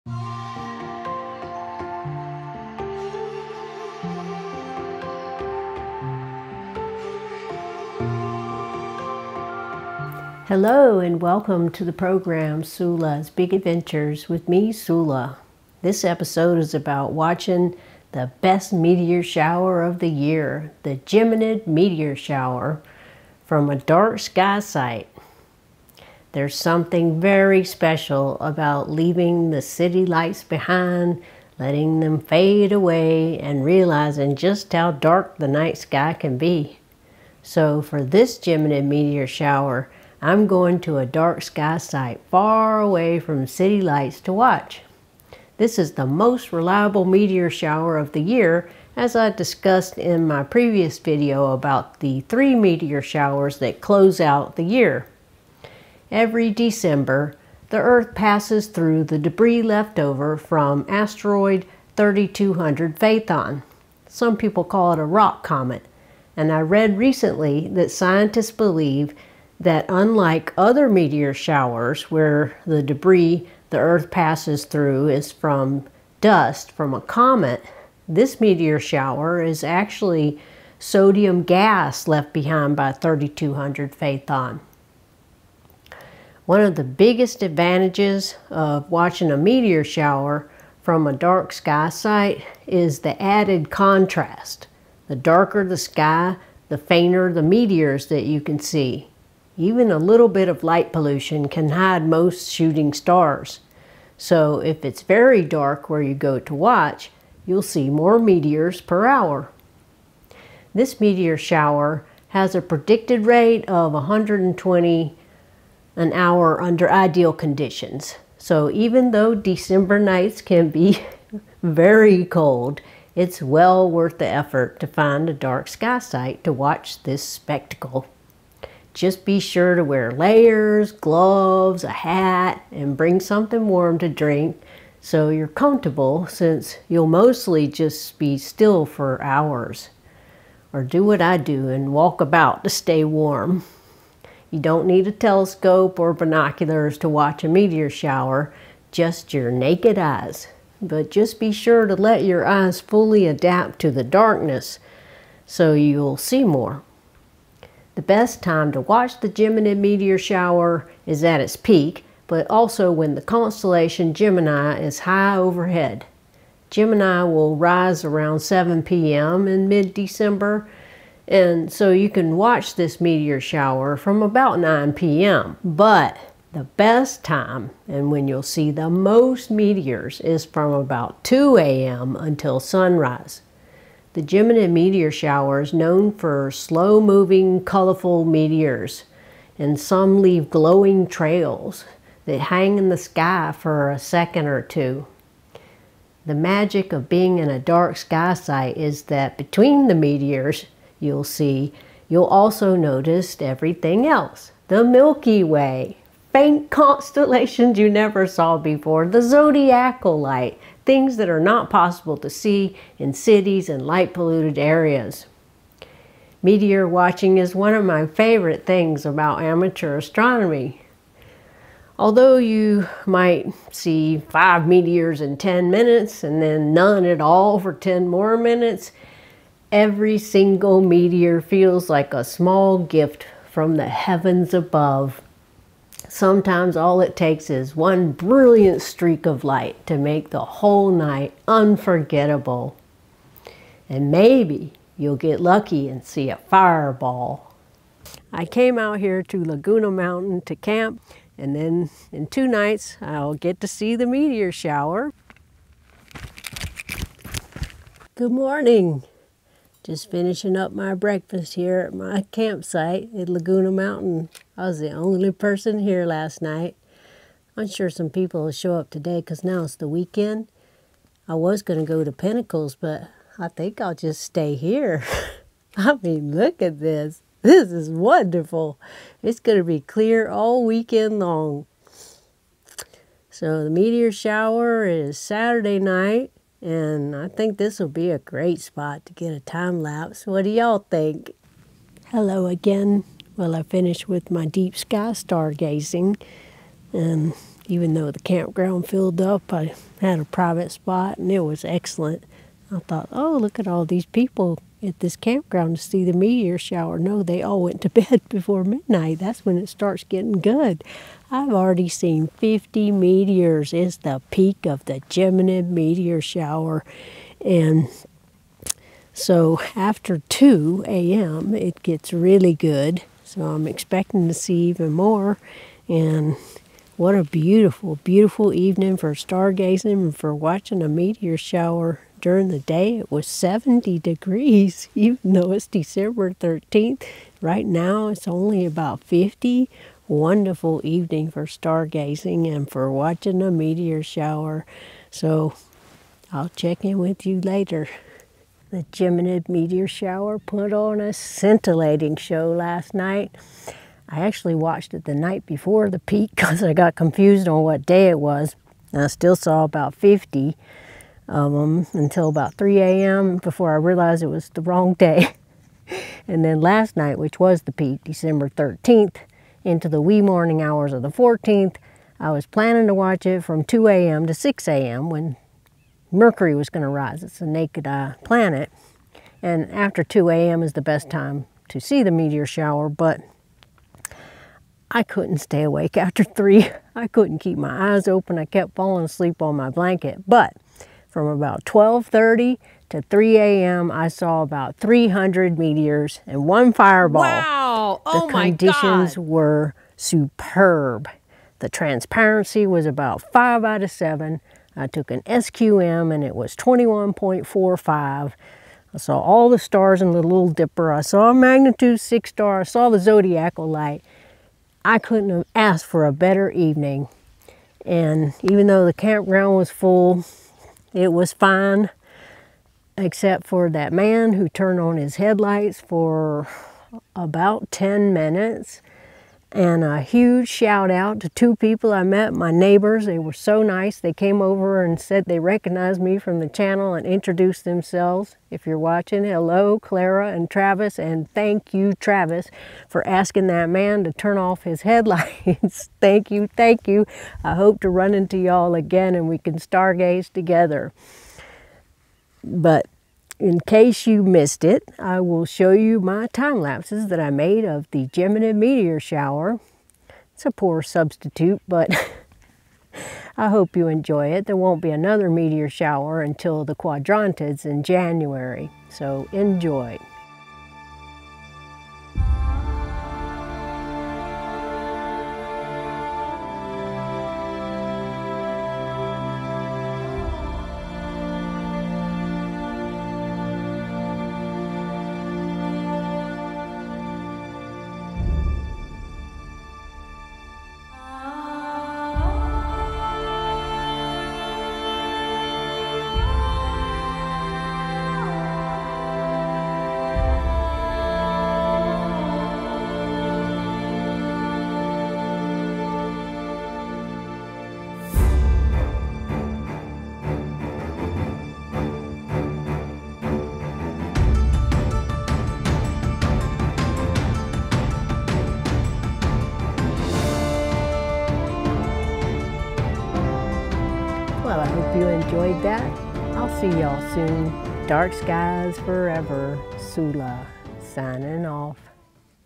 Watching the Geminid Meteor Shower Under Pristine Dark Skies
🌠 Geminid Meteor Shower from a Dark Sky Site 🌠 There’s nothing quite like watching the Geminid meteor shower under truly dark skies. In this video, I head to a remote dark sky site to experience one of the most reliable and spectacular meteor showers of the year. With minimal light pollution, the night sky comes alive as bright Geminids streak overhead, some leaving glowing trails that linger for seconds. Join me for a peaceful night of stargazing as I soak in the silence, scan the heavens, and share the awe of witnessing meteors burn up in Earth’s atmosphere. Whether you’re an astronomy enthusiast or just love the beauty of the night sky, I hope this video inspires you to look up and seek out darker skies for yourself. If you missed it or it was cloudy where you live, I hope you enjoy my timelapses. ✨ Clear skies and happy stargazing! ✨